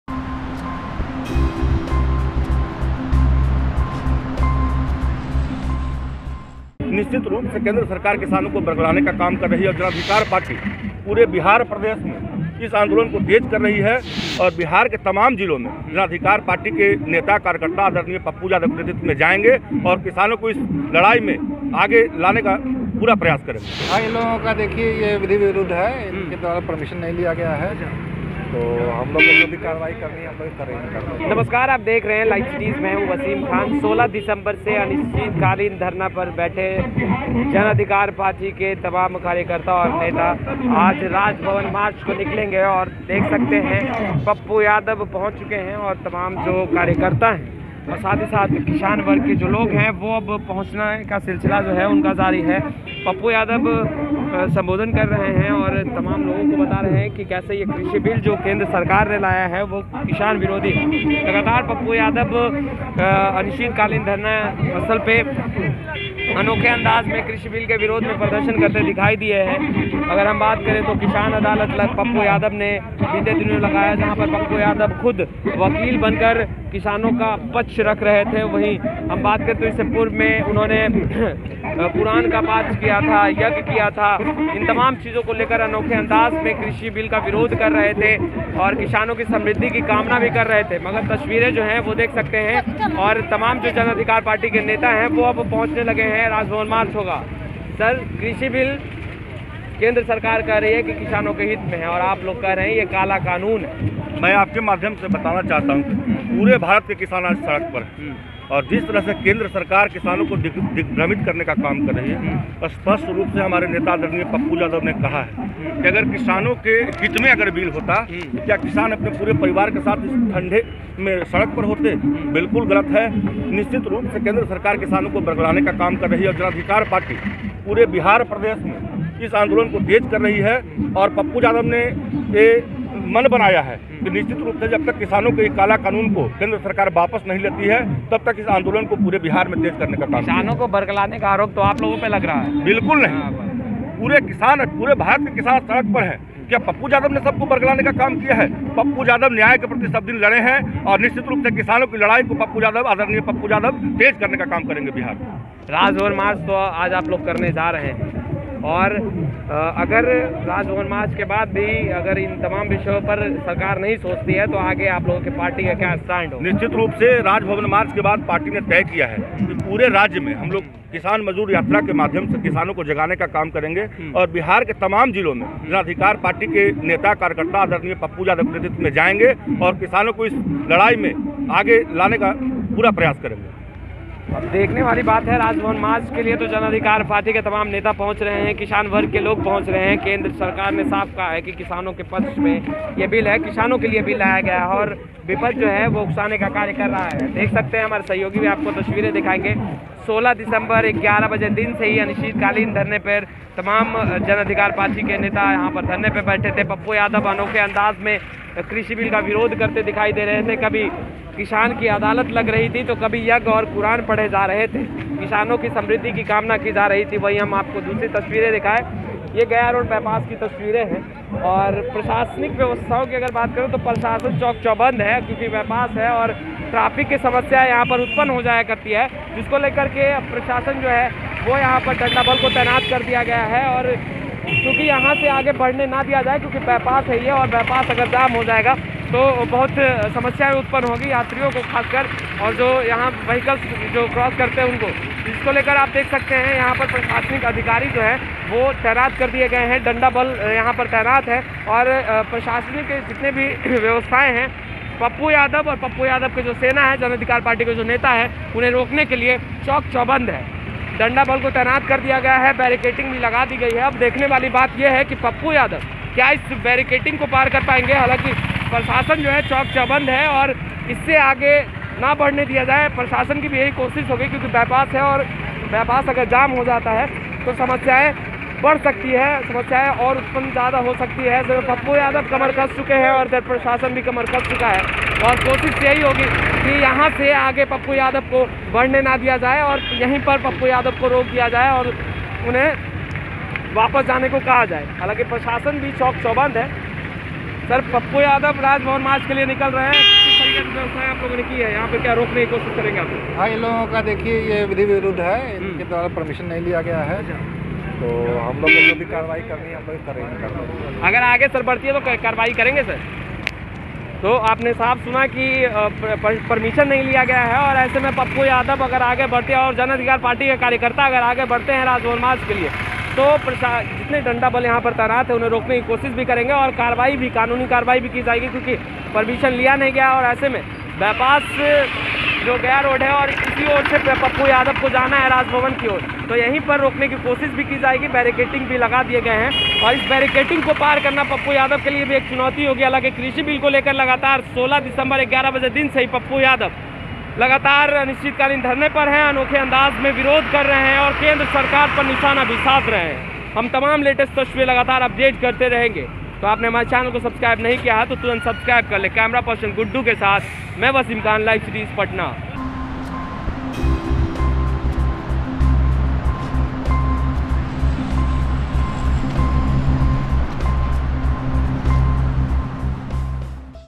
Kisan Andolan : Pappu Yadav का Raj Bhawan मार्च रोकने को एक्शन में प्रशासन, मिला ये जवाब
Kisan Andolan : Pappu Yadav का Raj Bhawan मार्च रोकने को एक्शन में प्रशासन, मिला ये जवाब | Bihar News | LiveCities